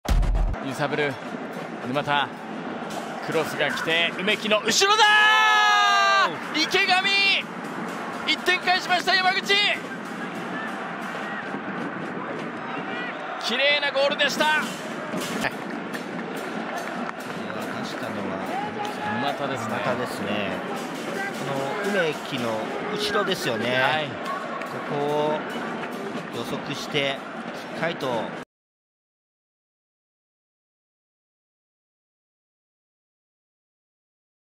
【GOAL】池上丈二(vs.大分トリニータ）クロスに飛び込み、今シーズン初ゴール
▼DAZNではJリーグ全試合をライブ配信！

・レノファ山口FC公式ファンクラブメンバー募集中！